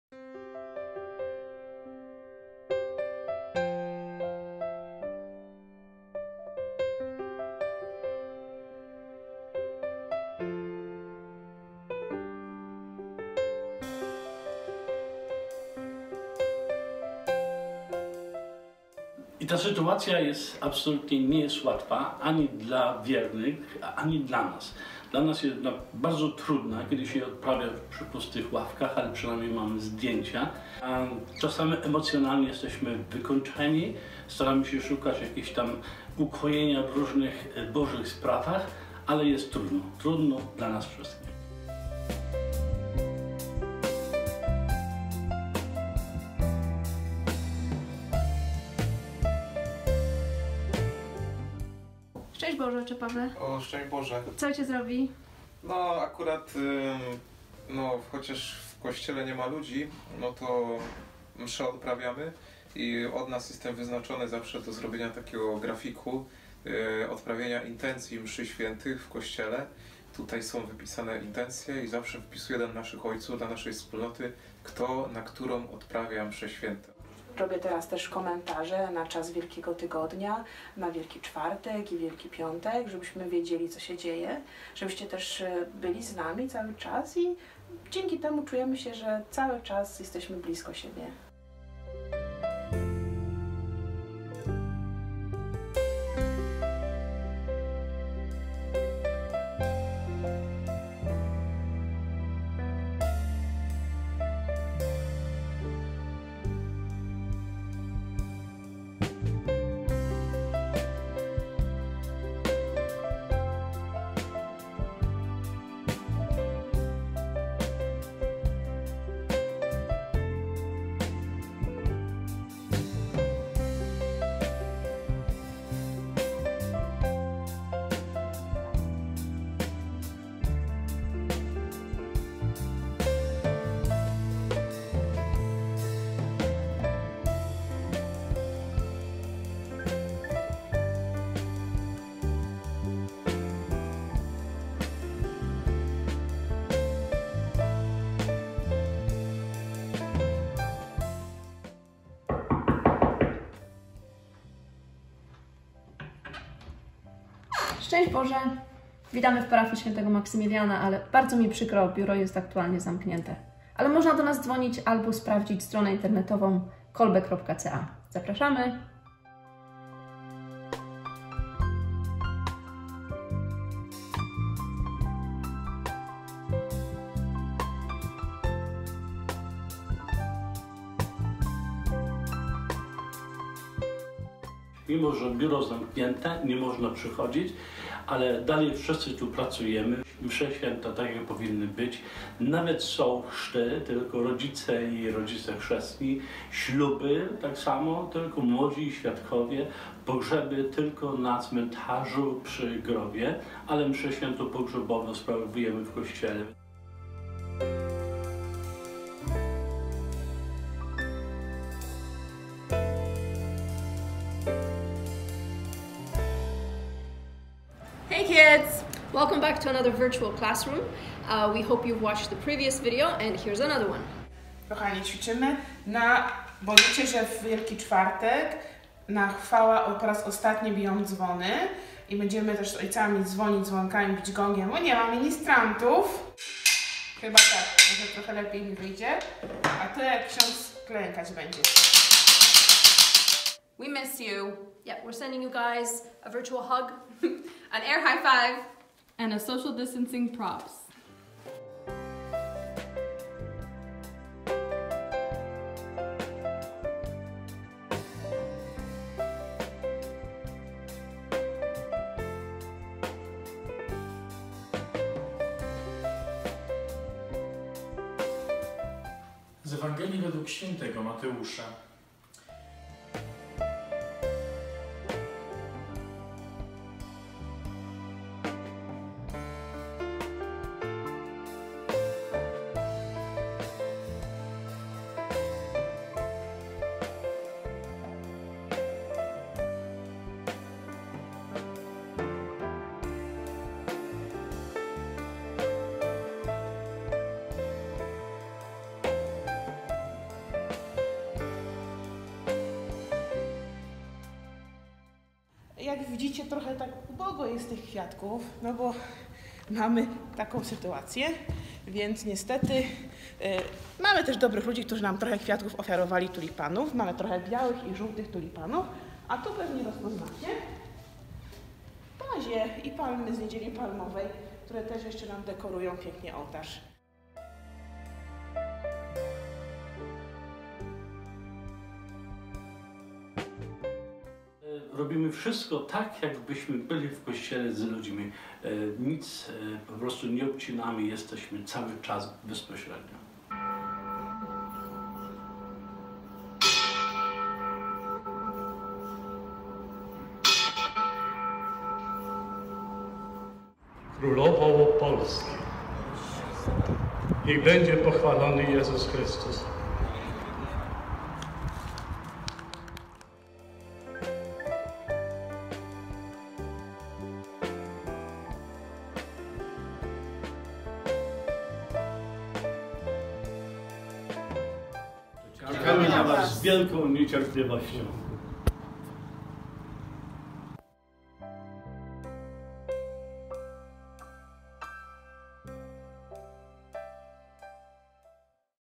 Za zamkniętymi drzwiami...
Kolbe Parish - dokument o życiu parafii, mimo drzwi zamkniętych...